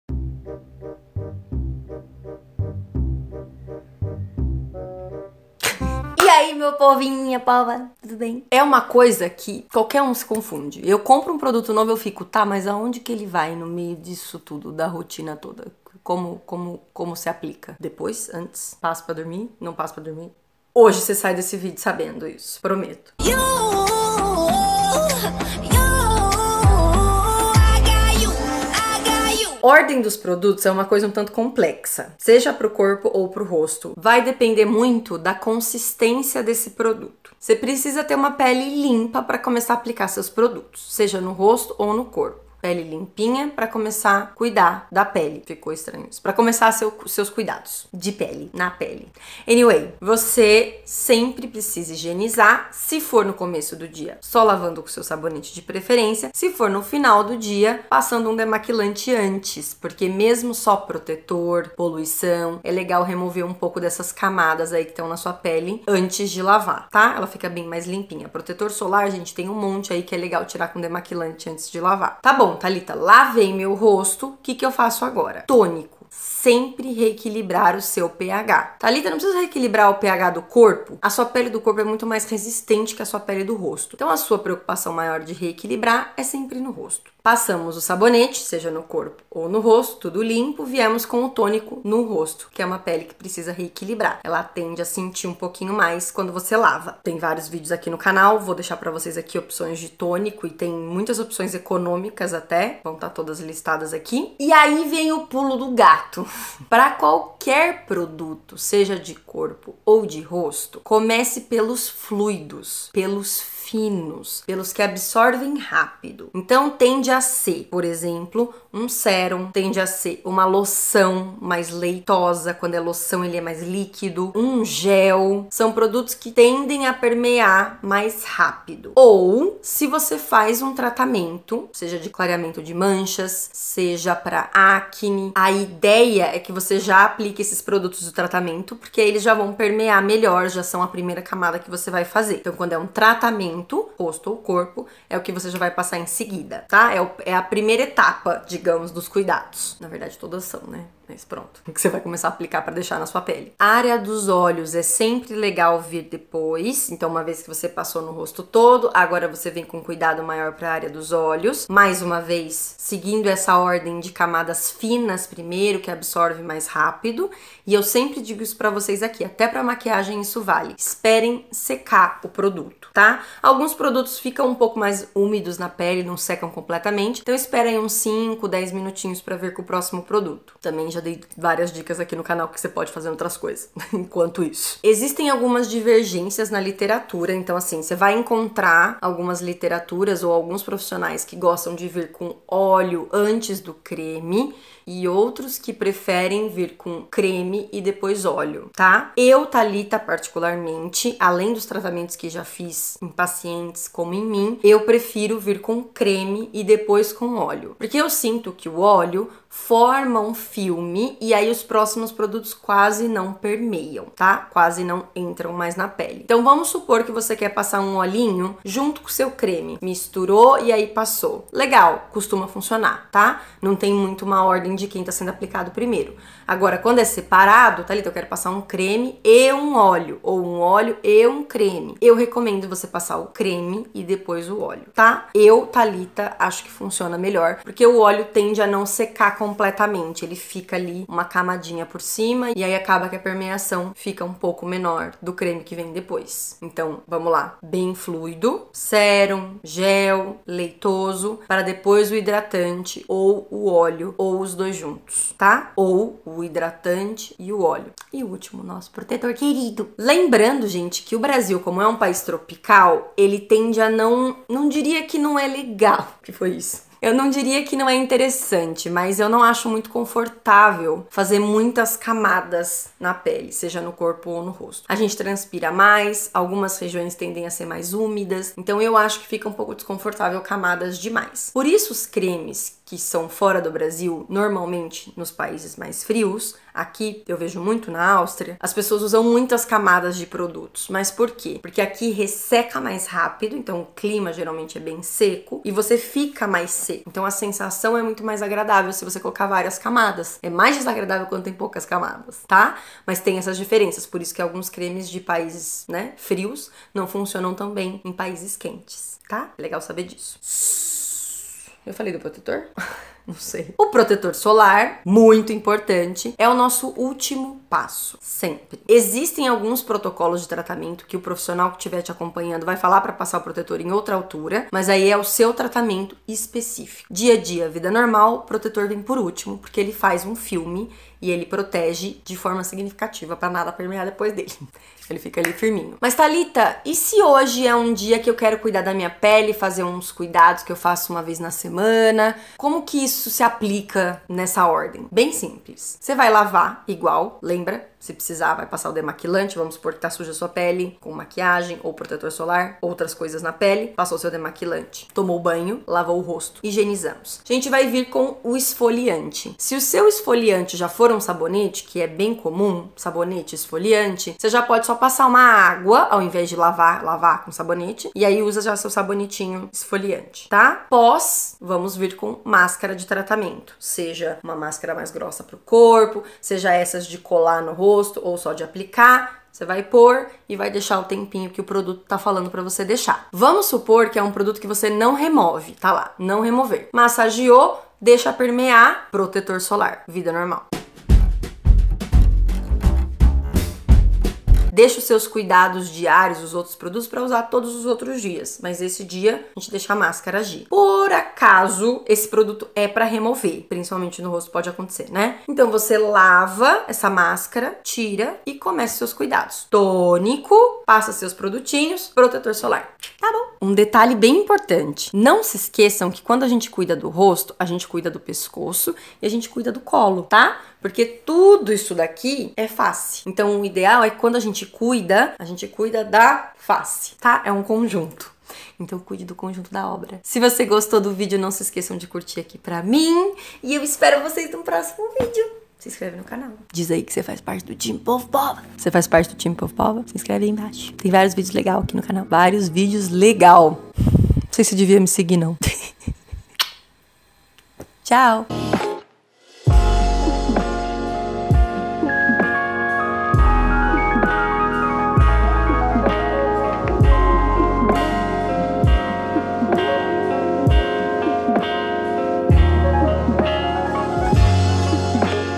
Ordem dos produtos para os cuidados com a pele | Corpo e rosto - Opinião de Esteticista
Pra gente se encontrar maaaaaais \o/

Pode me mandar um e-mail também, se quiser :)